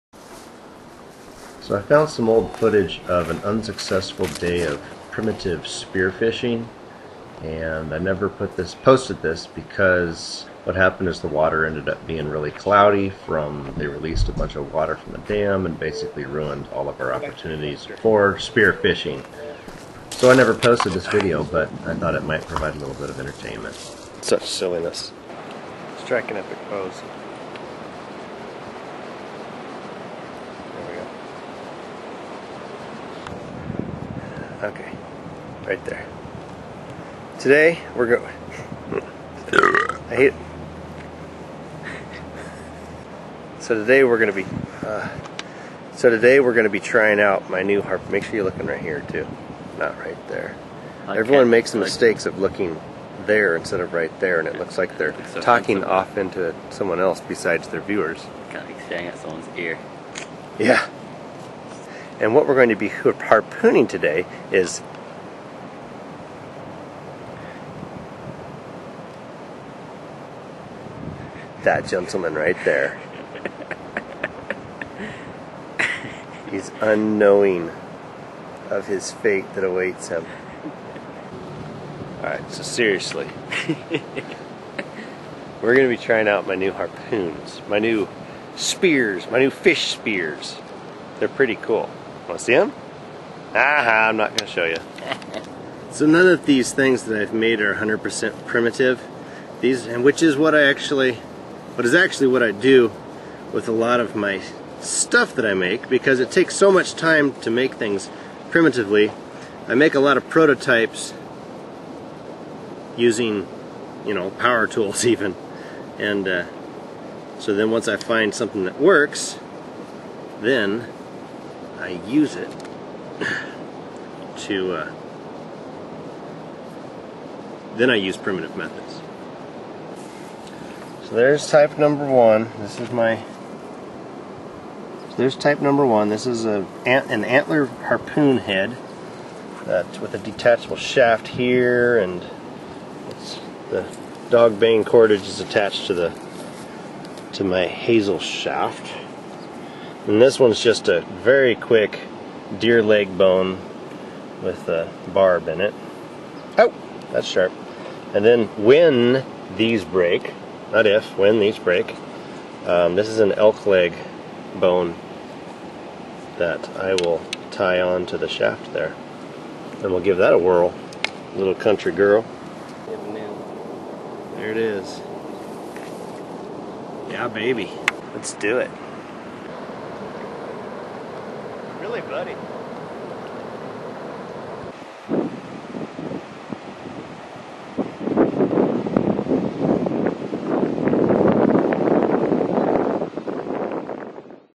Primitive Spear Fishing
I found some old footage from last summer. We were trying out a couple new fish spears on suckers but the water was too cloudy but I thought the video might be good for a few yucks..